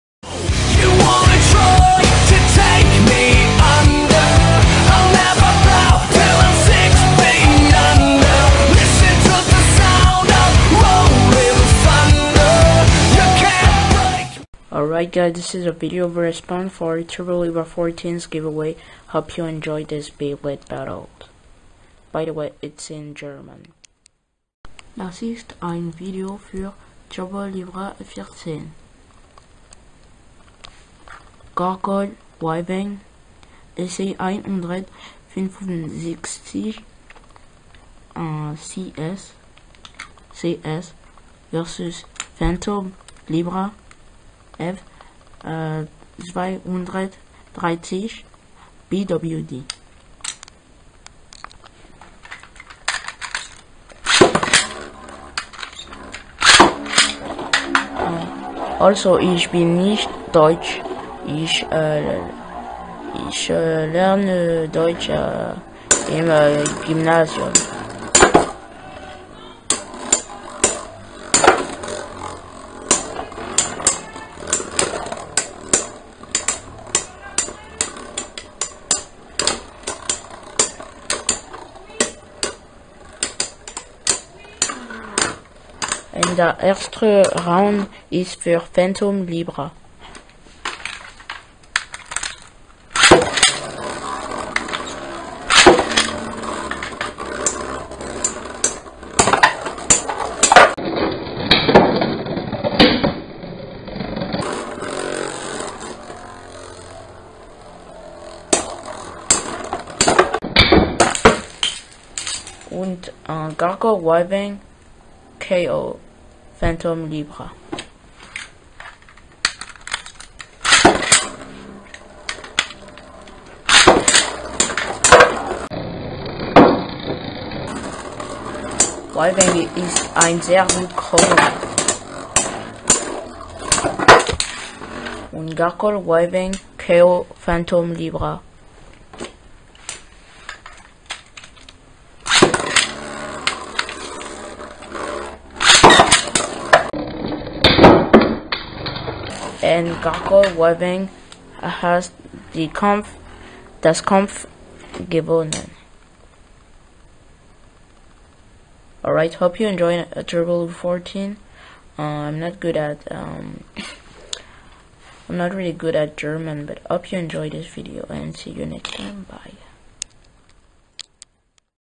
Gargole Wyvang SA165CS vs Phantom Libra F230BWD auf Deutsch ! TL14
█▄▀▄▀▄▀▄▀▄▀▄▀█  READ█▀▄▀▄▀▄▀▄▀▄▀▄█
This is a video for Germany's #1 Blader , The Owner of Gargole Wyvang SA165CS, one of my best rivals , the guys who loves TURTLES ! The awesome and Unique TurboLibra14 !!! because he is german i decided to do a video in his original language ! Hope you enjoyed !

Video Song : None
Intro Made by NL-Libra Blader